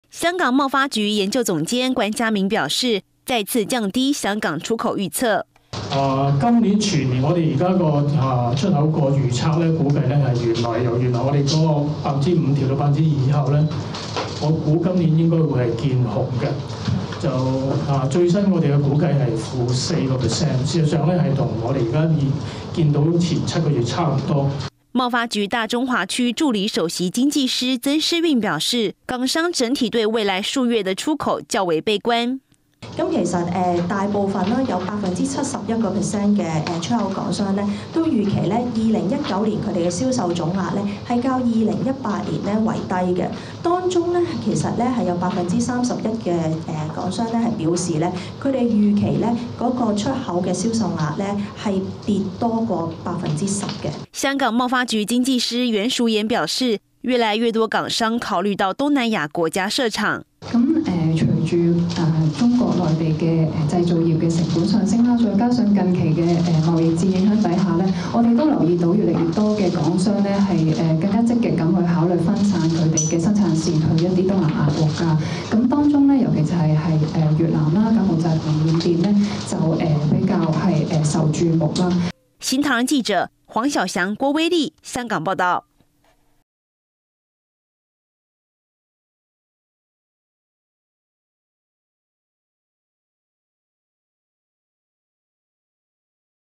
香港出口疲軟 今年預測降至負4%｜產業｜財經｜趨勢｜新唐人亞太電視20190924
香港貿發局表示，由於中美貿易戰加上環球經濟增長放緩，香港出口表現疲弱，並再度降低今年出口預測，預料全年出口數值將減少4%。

支持好媒體！歡迎加入新唐人之友
 ♛ 「訂閱」本頻道